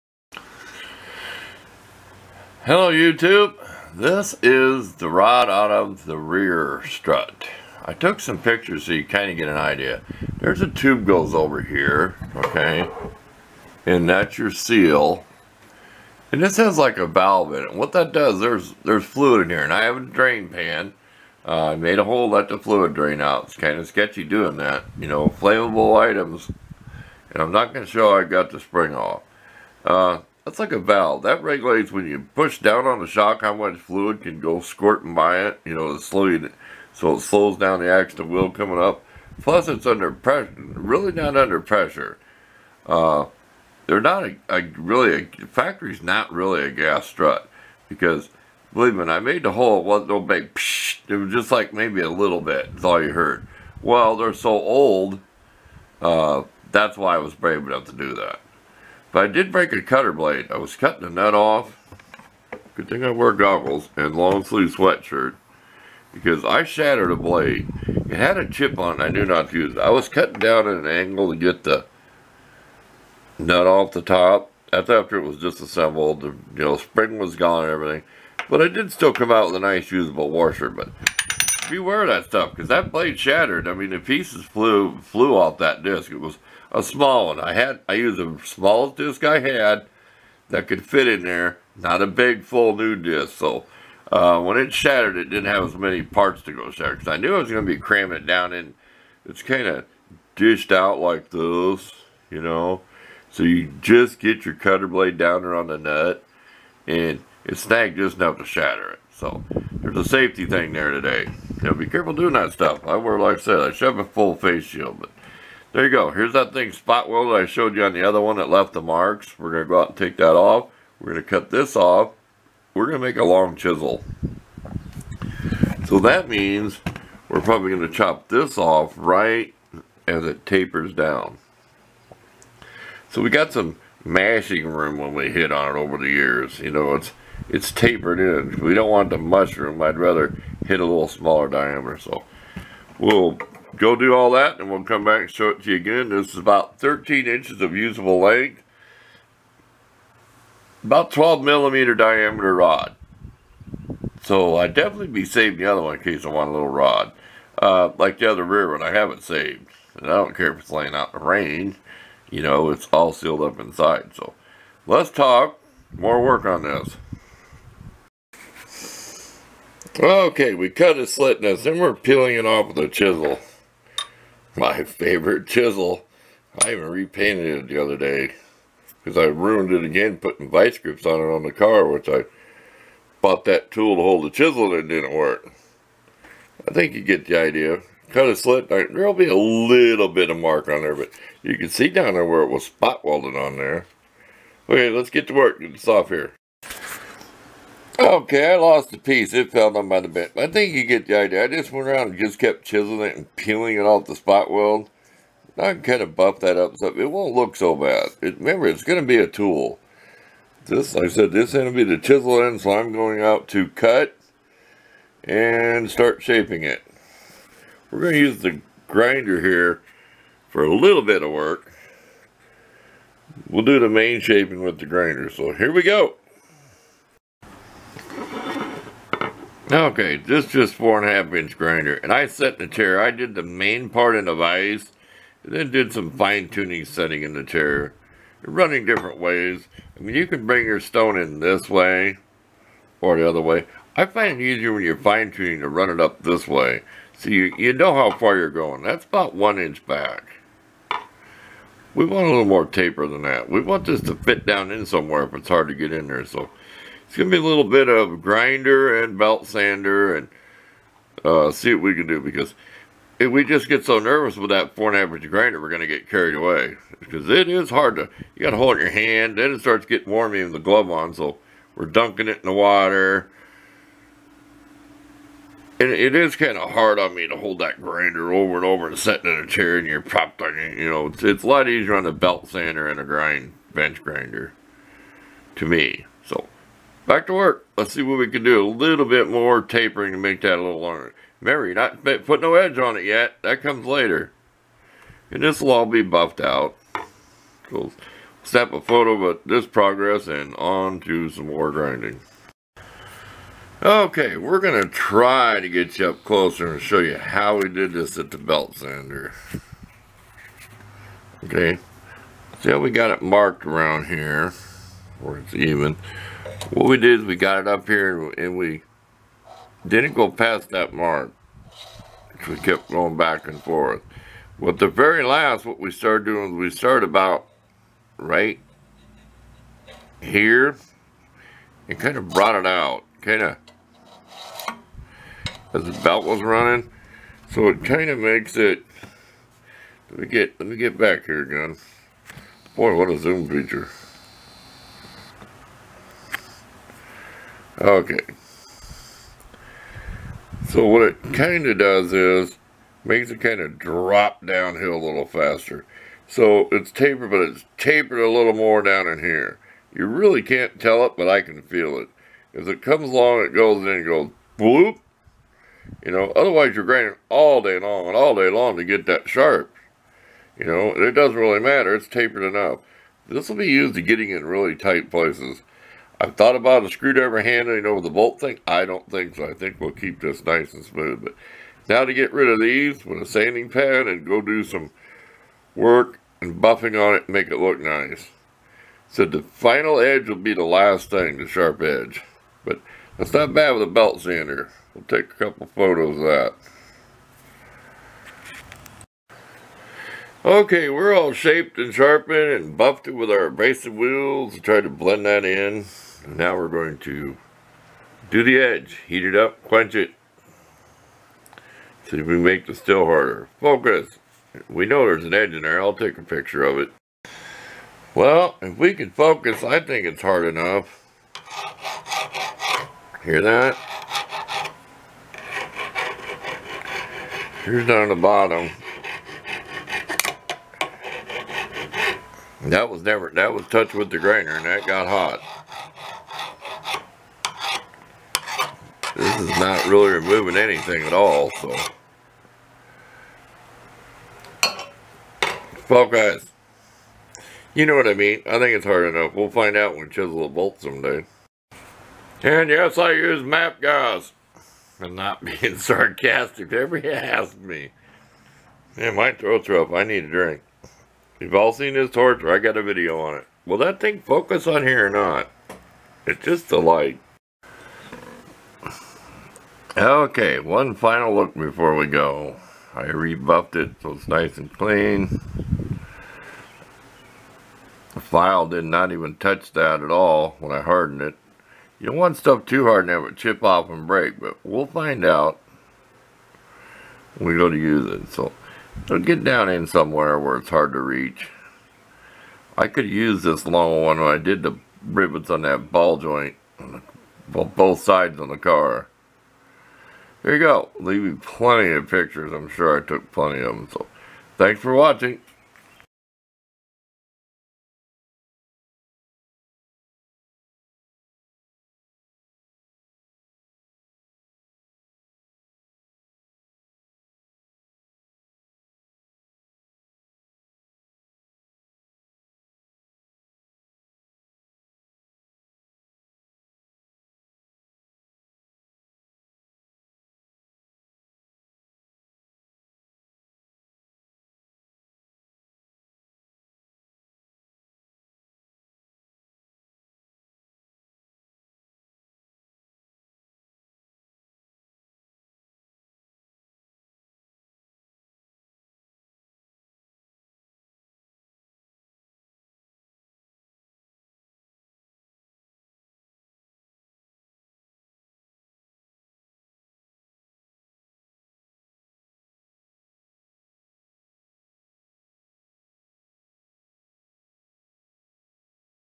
HOMEMADE CHISEL made from a 12 mm Dia. Shaft from a Car Strut
About 1 foot long hard steel shaft that was salvaged from a rear strut from a car Warning disassemble a car strut at your own risk the shock can have pressure and the coil spring is under tension Note : I do not get paid to show any products on my videos